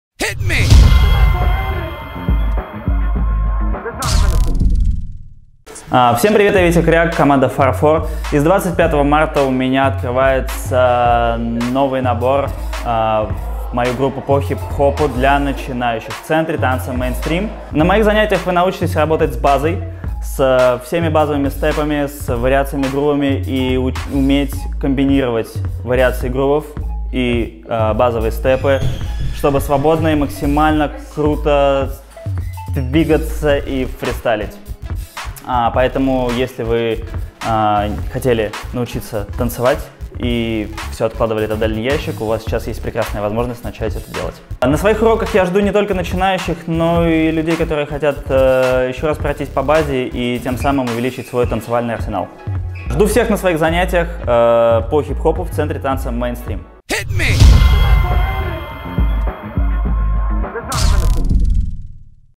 Новый набор в группу Вити Кряка - Hip Hop Basics в центре танца MAINSTREAM
Новый набор!
Группа HIP HOP BASICS
в центре танца MAINSTREAM

Преподаватель: Витя Кряк ( команда Far For )

Москва, Кривоколенный пер. 14